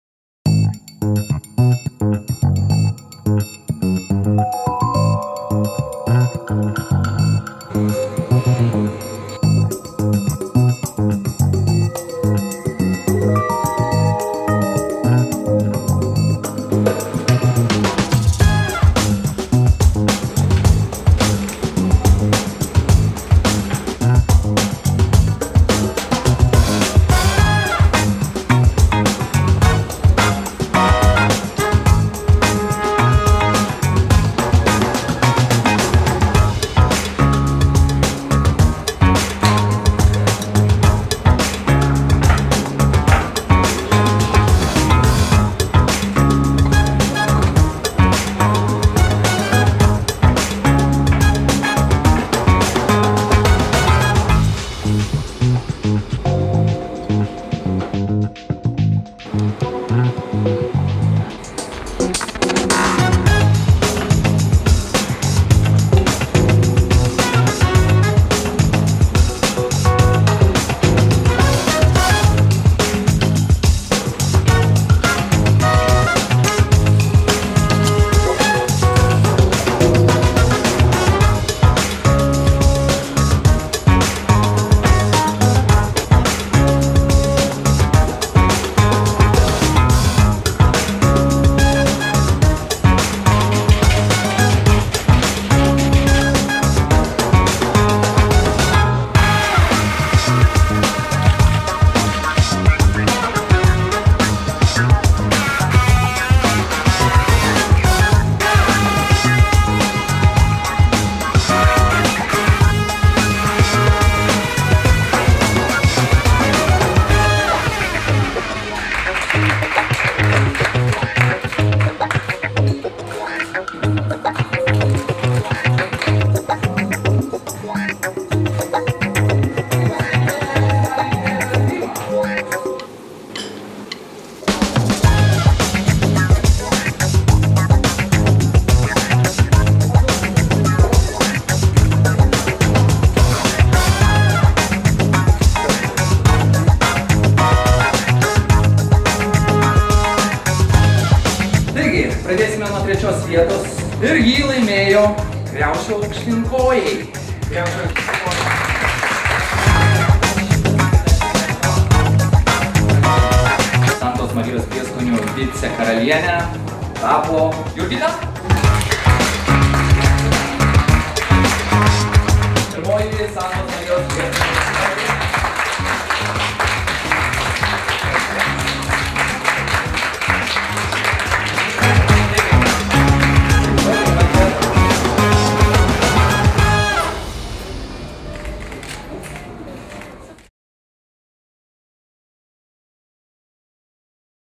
"Santa Maria" prieskonių karaliaus/karalienės rinkimų finalas / 2013 05 06
Gegužės 6-osios, Tarptautinės dienos be dietų, vakarą, išrinkta pirmoji Lietuvoje Prieskonių karalienė. Dėl šio titulo „Santa Maria" inicijuotame konkurse rungėsi trys kulinarijos entuziastės, pakeliui į finalą įveikusios 50 konkurso dalyvių. Finalisčių pasirodymą ir jų pagamintą vakarienę vertino virtuvės virtuozai Deivydas Praspaliauskas ir Tomas Rimydis, kulinarinių knygų vertėja, maisto tinklaraščio autorė Eglė Mačerauskienė ir „Santa Maria" prieskonių ekspertas Artūras Braslauskas. Nugalėtoja tapo ir prieskonių karūna pasidabino kaunietė Vita Klišaitienė!